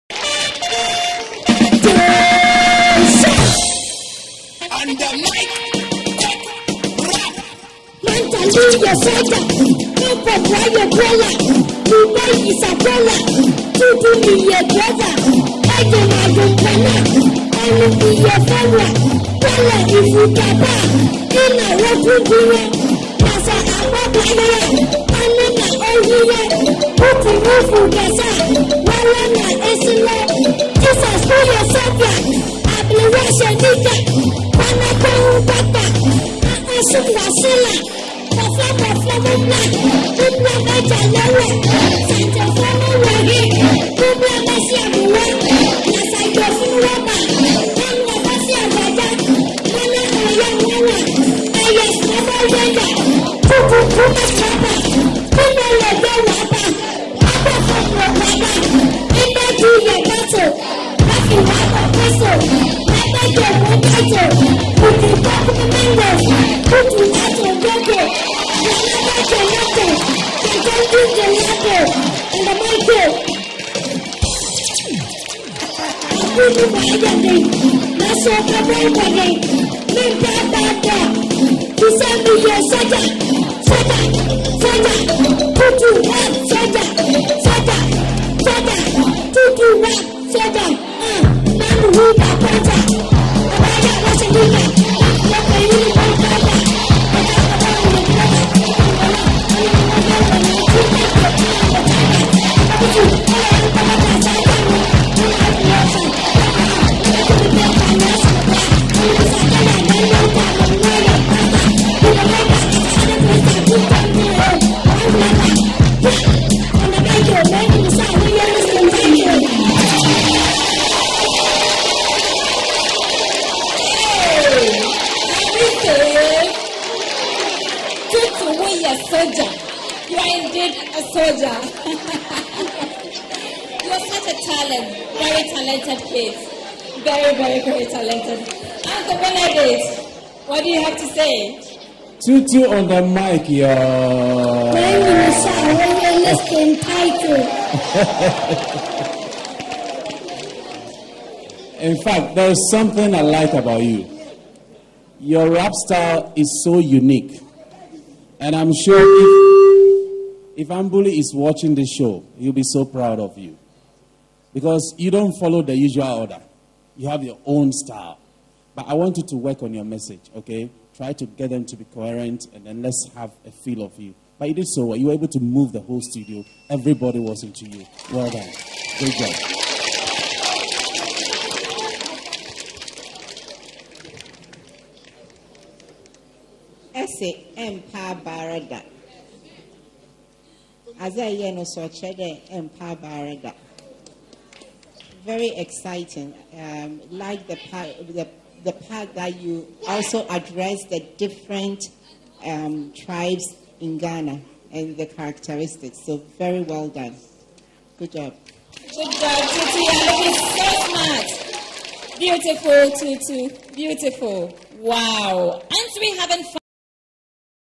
tutulapato on tv3
youngest rapper in ghana kills it on tv 3